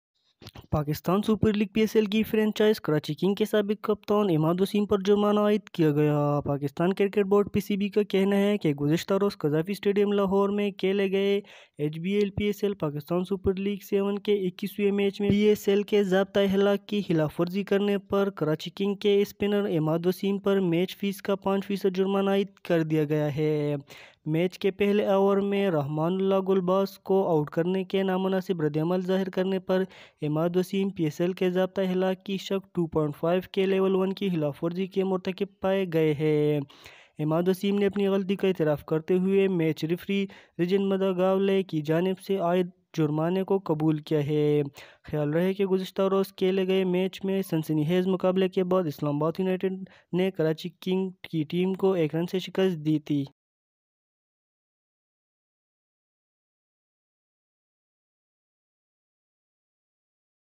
Imad Wasim Fined For PSL Code Of Conduct Celebration
In This Video I Will Tell You that